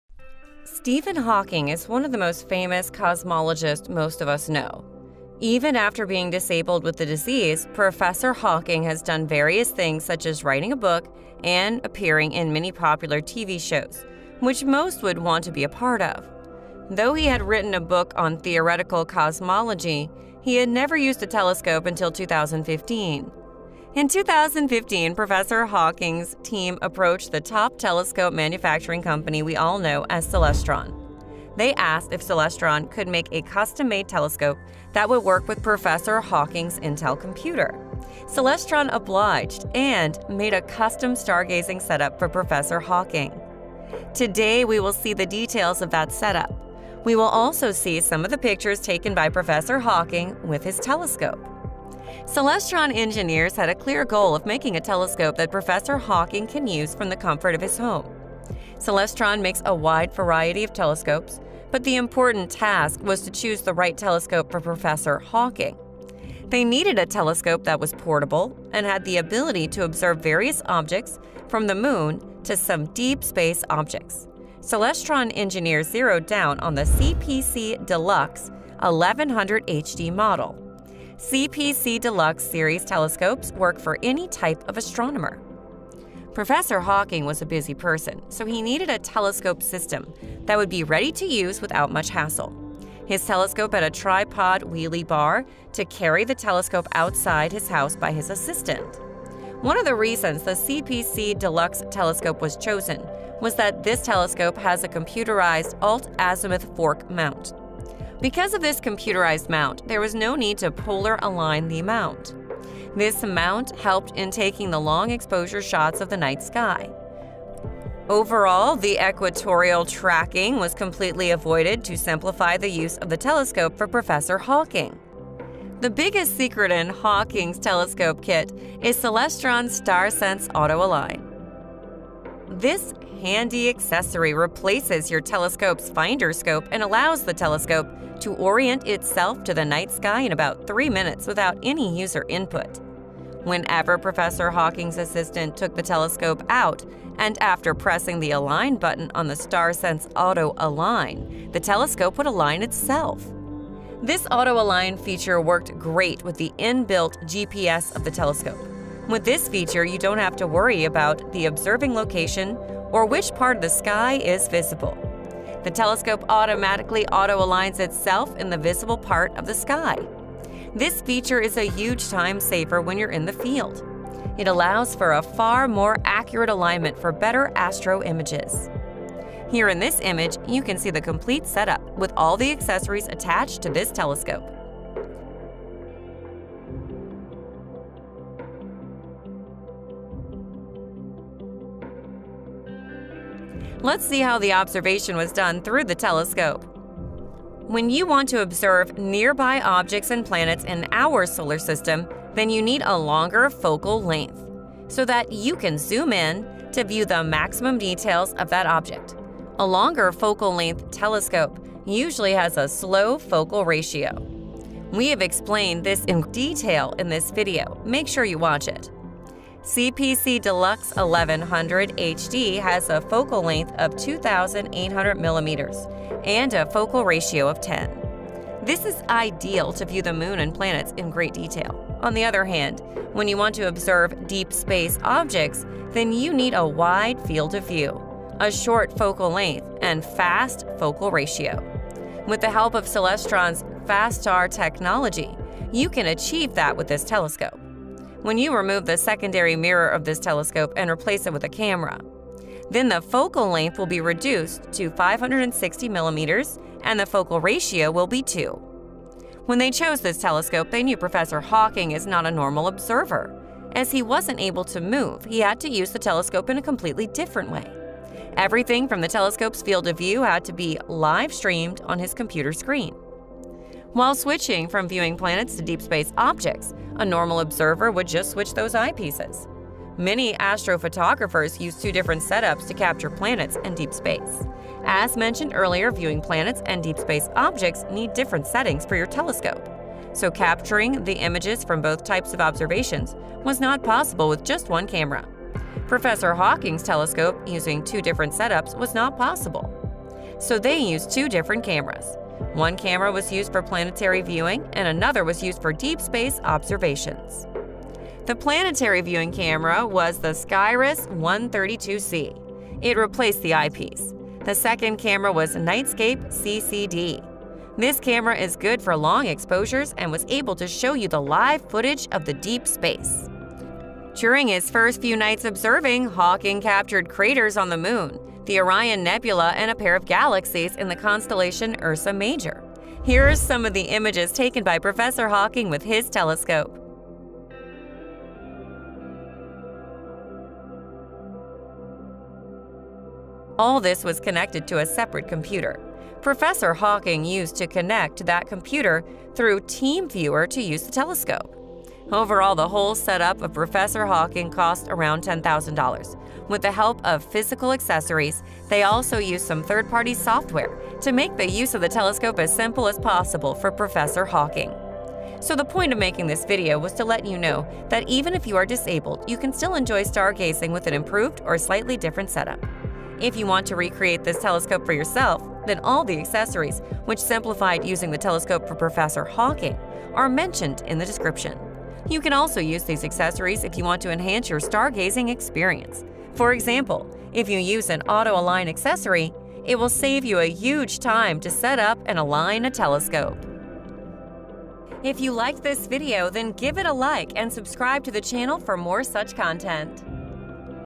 Stephen Hawking's Telescope Captures Stunning Views of the Universe
Stephen Hawking was the greatest mind. His theories are getting proved one by one. Professor Hawking had a telescope from which he used to observe the cosmos. In this video, we explained the details & how the setup of his telescope was built.

1. Celestron CPC Deluxe 1100 HD
2. Celestron CPC Deluxe 800 HD
3. StarSense Auto-Align
4. Celestron Nightscape CCD Camera
5. Skyris 132C Camera
6. Celestron 12VDC 5A
7. UpBright 12V 10A 120W
8. VCT - 220V/240V AC 13A Universal
9. Rosewill USB Hub with 4 Port USB 3.0 USB
10. Wheeley Bars Telescope Wheels
11. Dell Inspiron Laptop
12. Tripp Lite Keyspan High-Speed USB to Serial Adapter
13. Software Bisque’s TheSkyX Pro with Camera Control Plugin